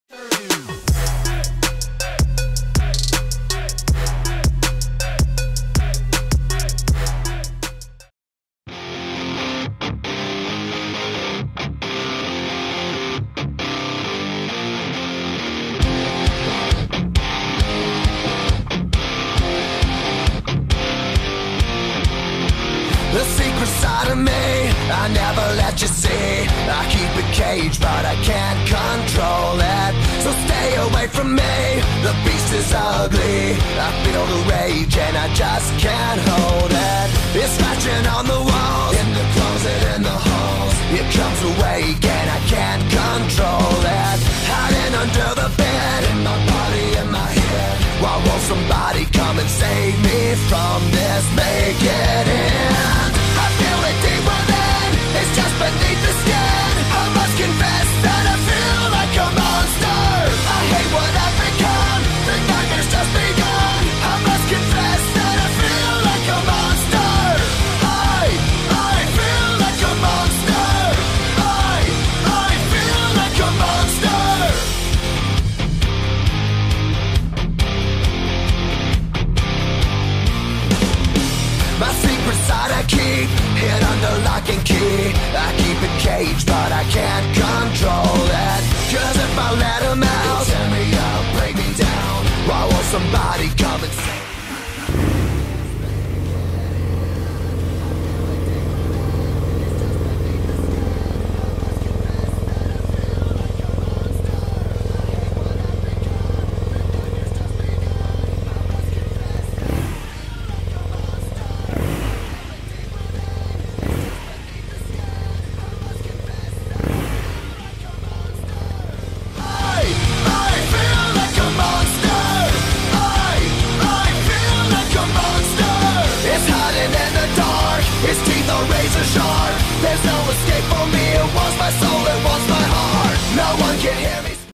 REVIEW 2018 BMW R9T URBAN GS
BMW R9T Urban GS registrasi 2018 ( nik 2017 )  FULL PAPER  plat B ( Jakarta ) ATPM Maxindo , pajak terima panjang  05. 20, odo 6200 km , asesoris :

Engine guards, Windshield, Handguard Wunderluch, Bracket belakang, Ban tahu Continental

1200 cc mesin Boxer ABS, Traction control, Penggerak gardan